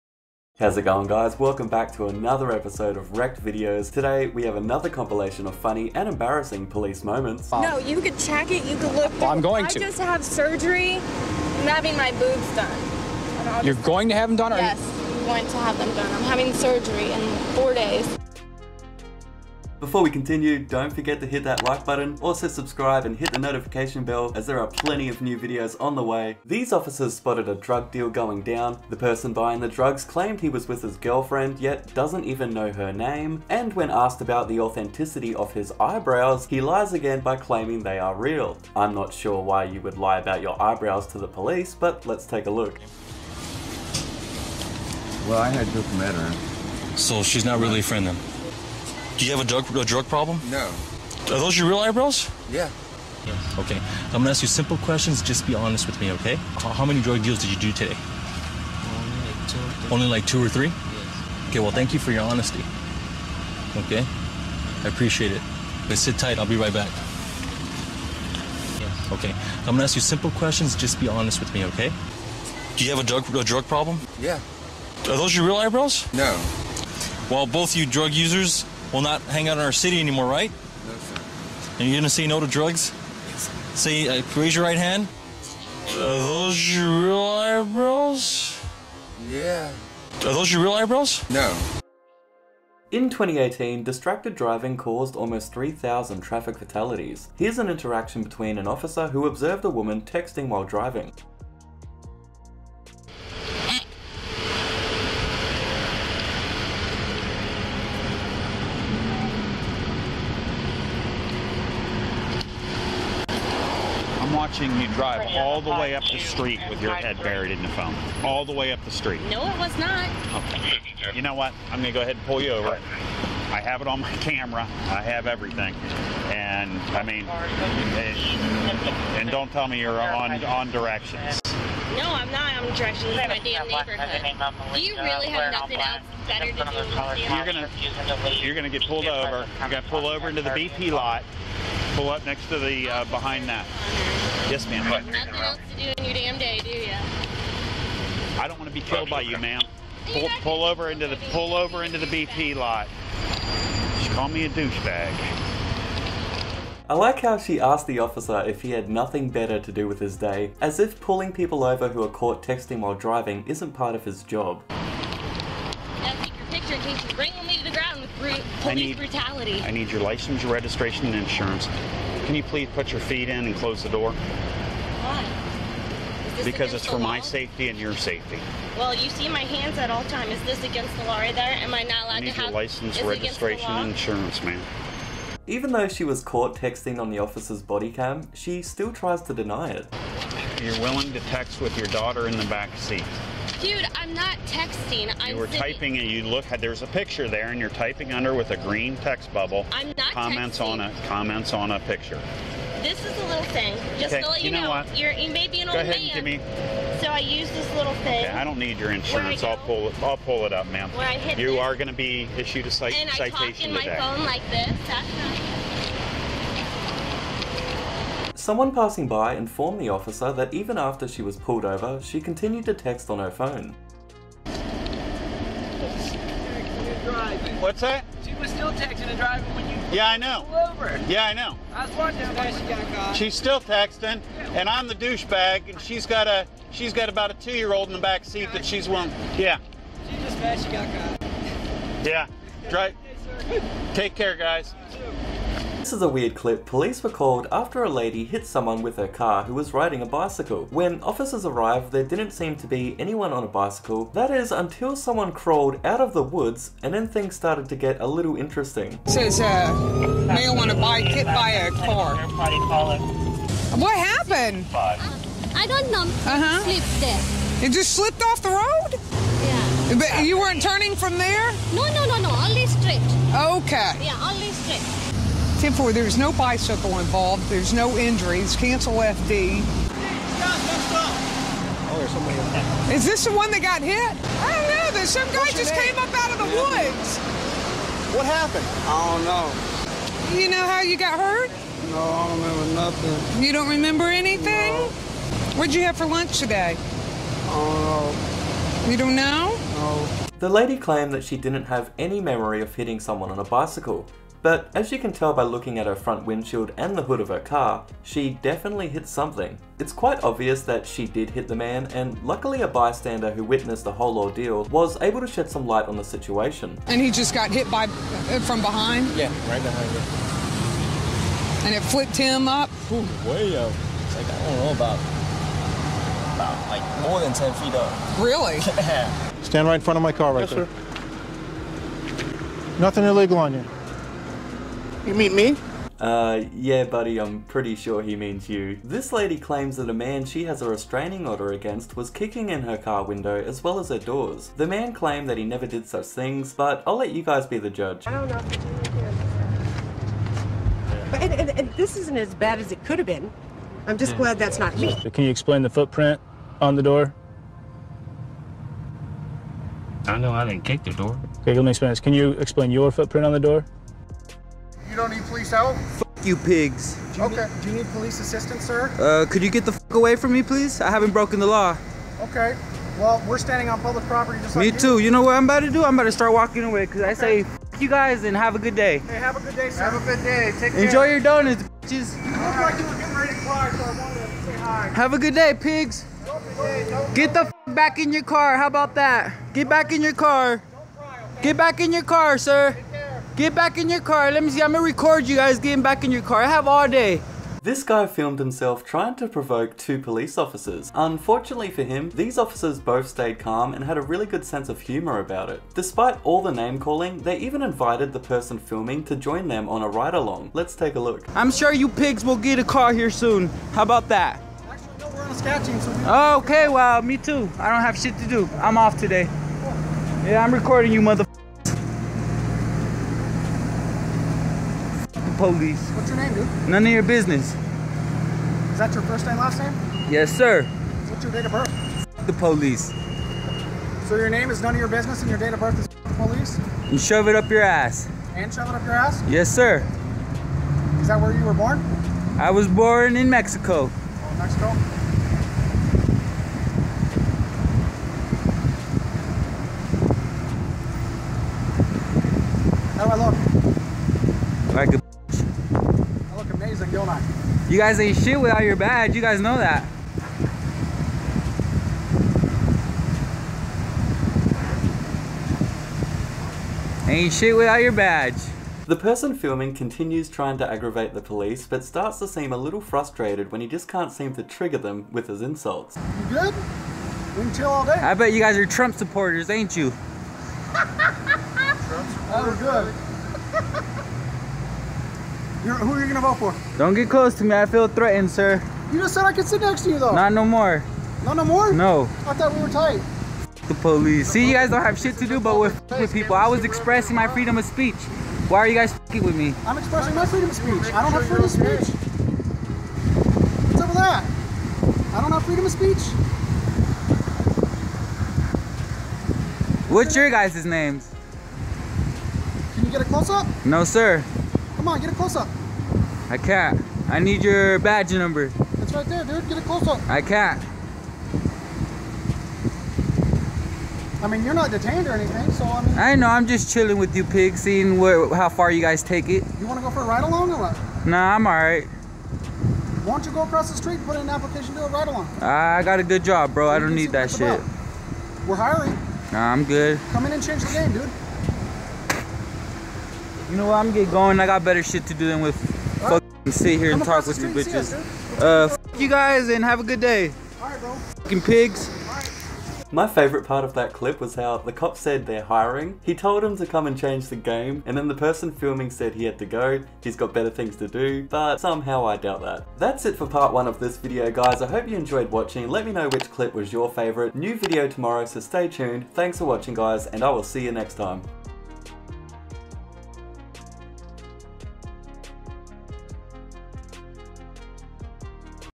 25 FUNNIEST AND MOST EMBARRASSING POLICE MOMENTS
25 FUNNIEST AND MOST EMBARRASSING POLICE MOMENTS
This video highlights funniest Police moments with more Instant Justice